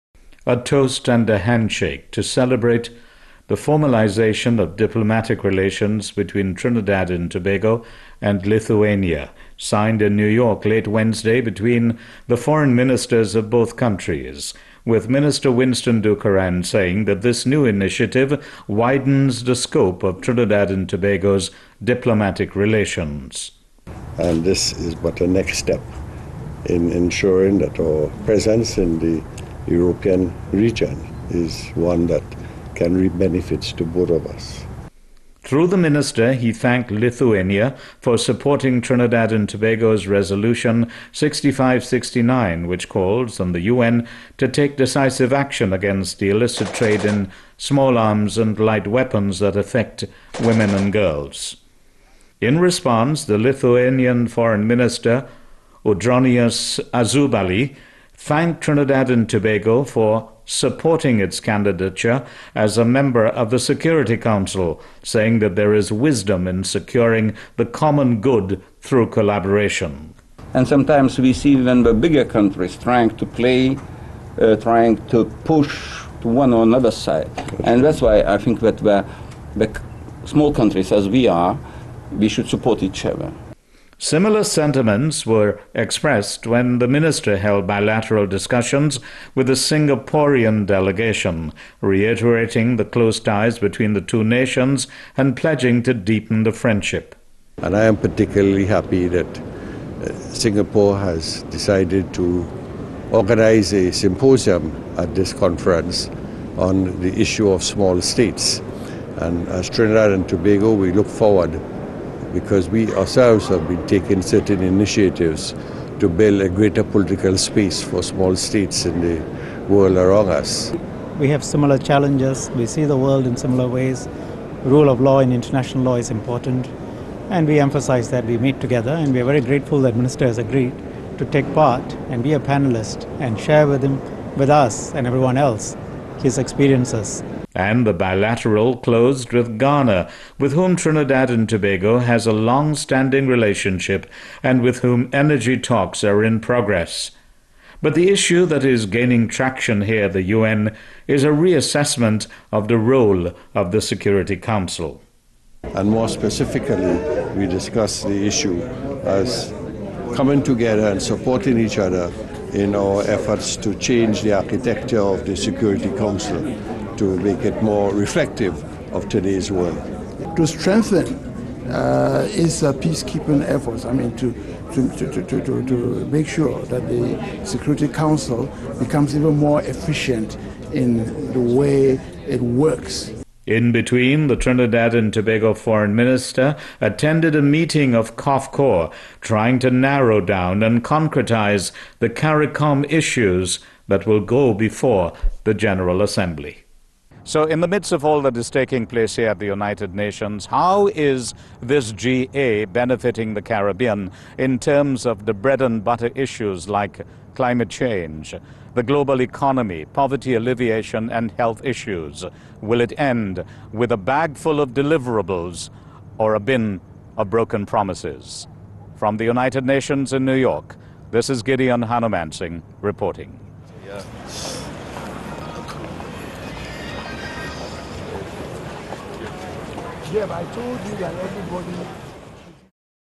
Day 3 for Minister Dookeran at UN General Assembly
Minster Dookeran formalized Diplomatic Relations with Lithuania, signed late on Wednesday between the Foreign Minister of Lithuania, Audronius Ažubalis & Minister Dookeran.

The Minister also had a bilateral discussions with Singapore's Foreign Affairs Minister, Prof. Jayakumar Shunmugam and Minister for Foreign Affairs and Regional Integration of Ghana, Muhammad Mumuni.

The Minister also attended a meeting with COFCOR to narrow down  & concretize the CARICOM issues that will go before the General Assembly.